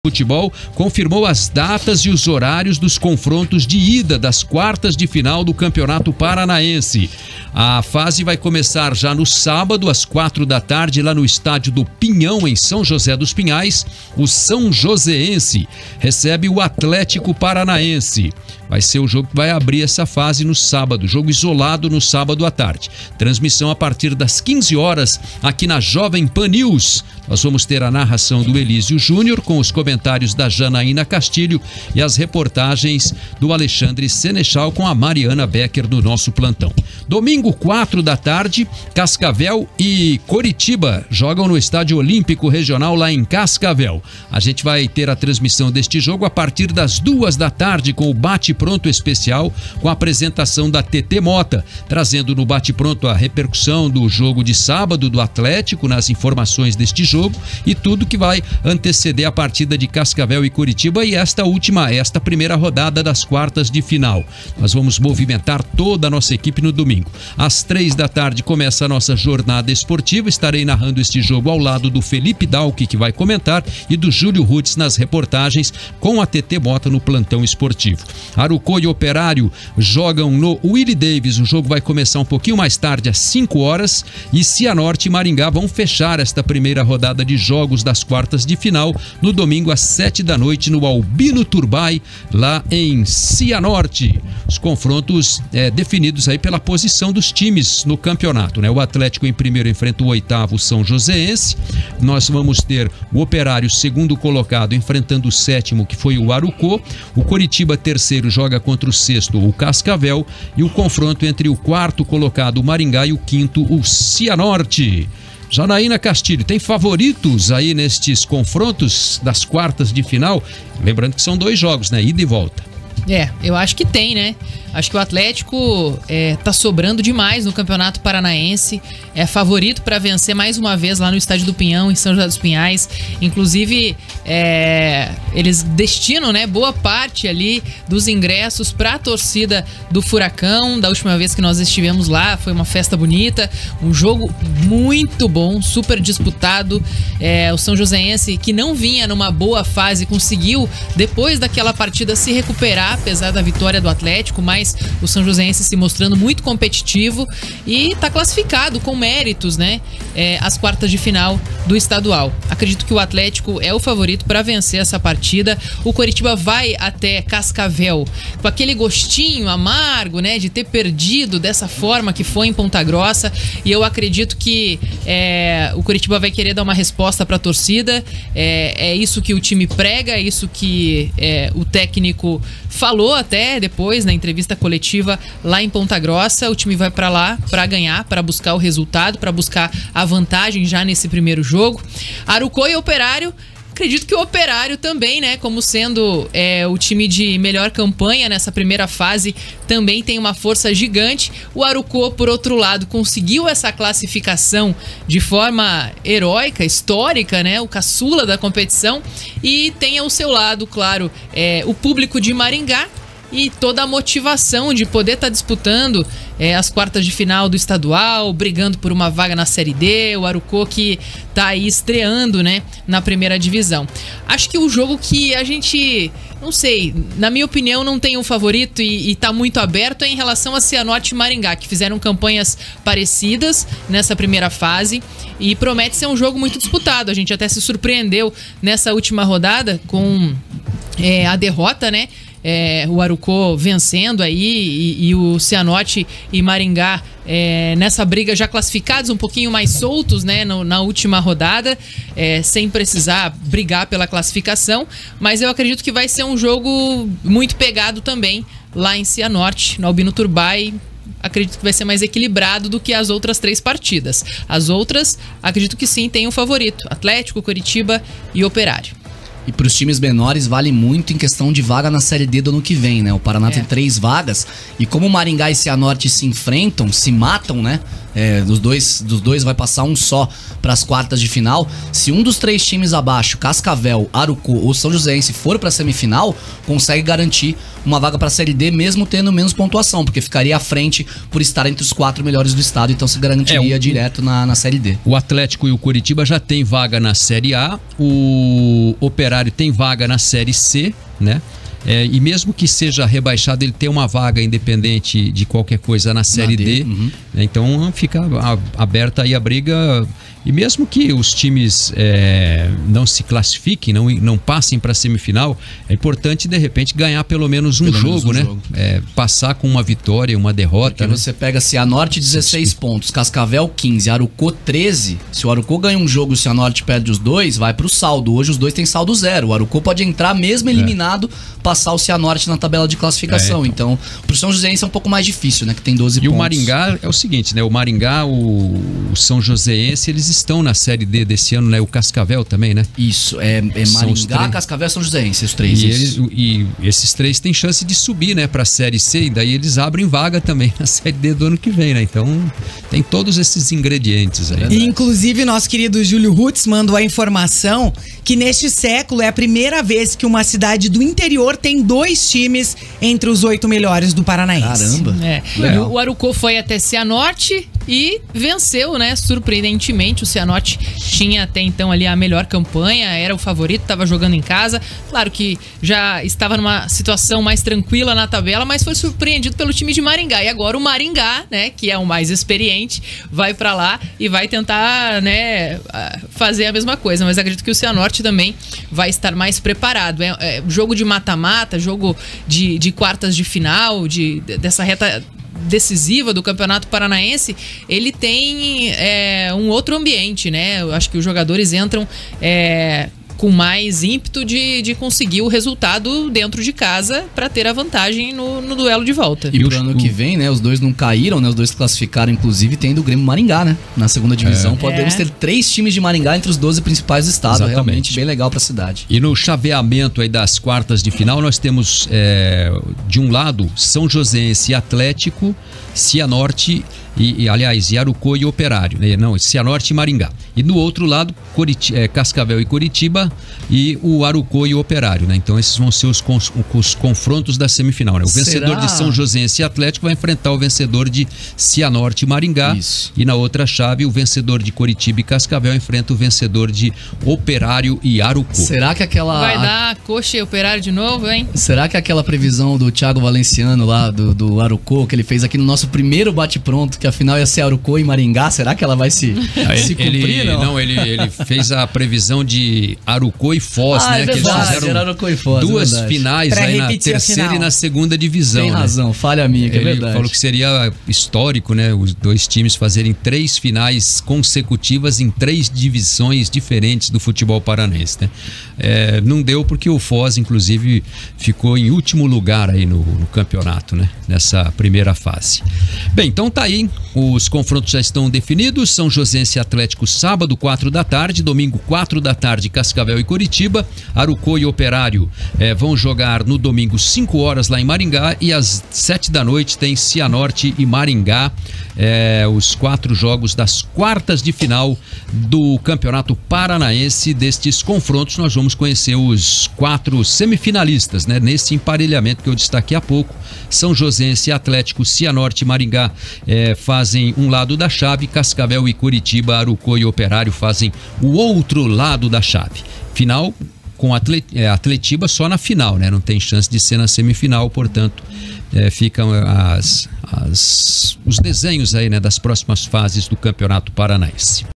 Federação Paranaense de Futebol define datas e horários das quartas de finais
A FPF confirmou as datas e os horários dos confrontos válidos pelas quartas de final do campeonato paranaense.